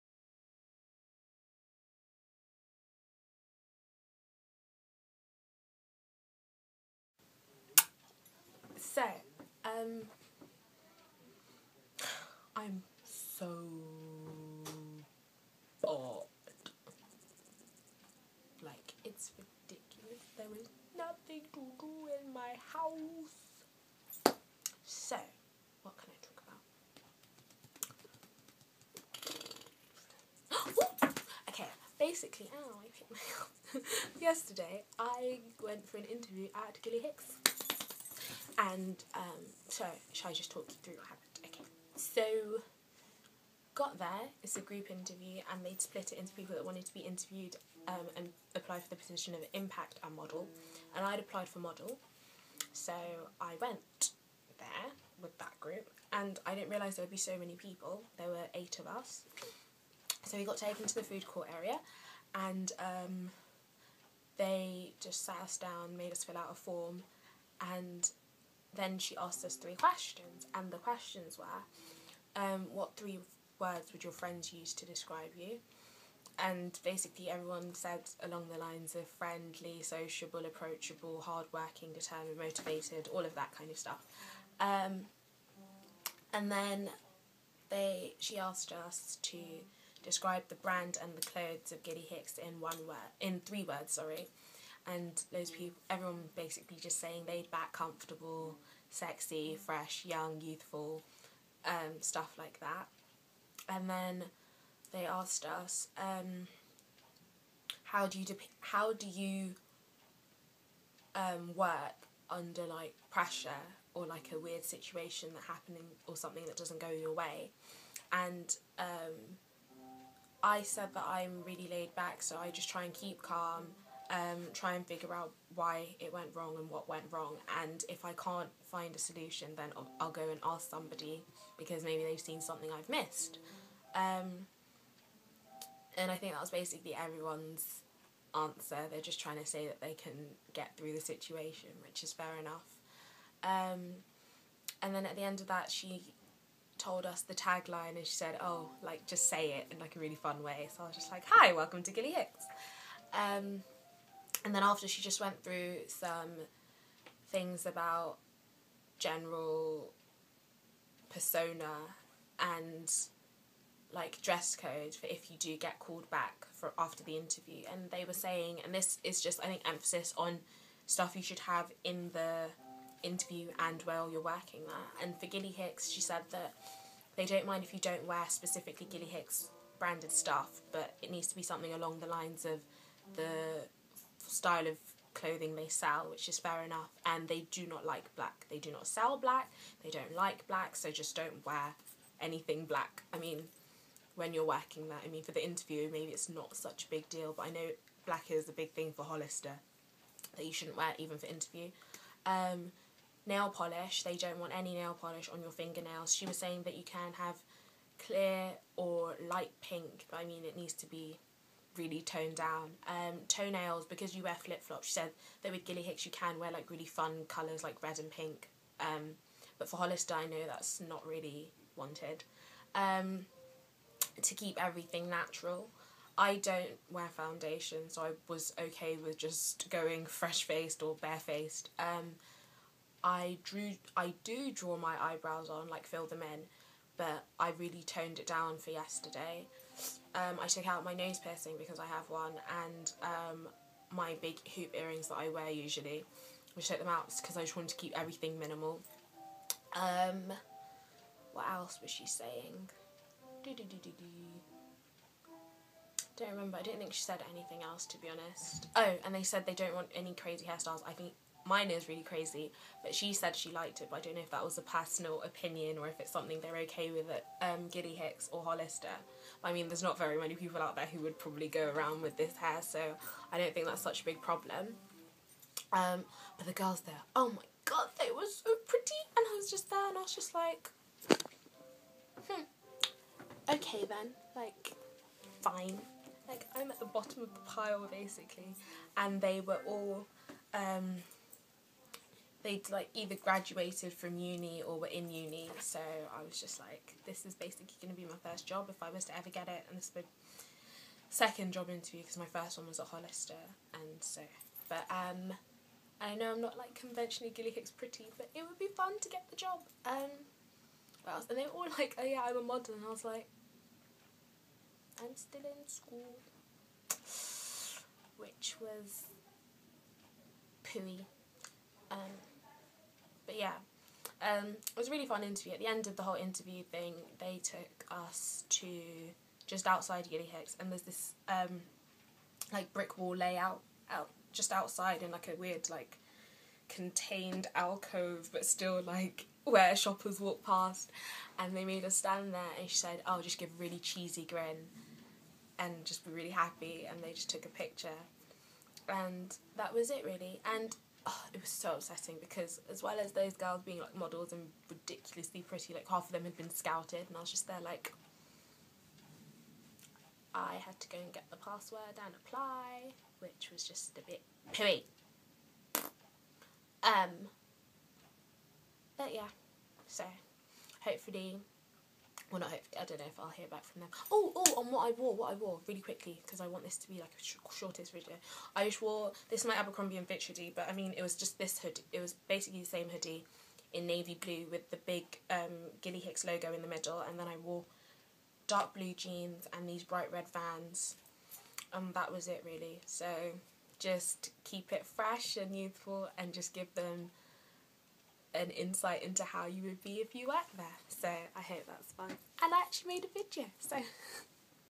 Gilly Hicks Interview (Abercrombie/Holliser general)
How my Gilly Hicks interview went and some pointers for Abercrombie, Hollister and Gilly Hicks interviews in general.